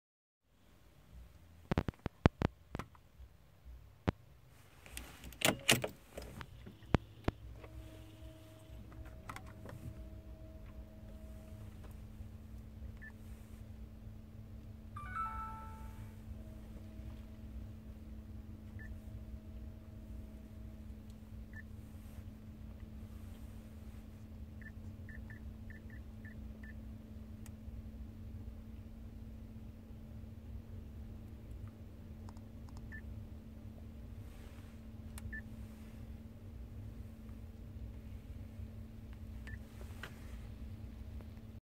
Volkswagen Touareg RCD 550 webasto activation (auxiliary heating)
Please support my website with one click. It is a dental office website. Thank you!🙏

How to activate auxiliary heating on RCD 550 touareg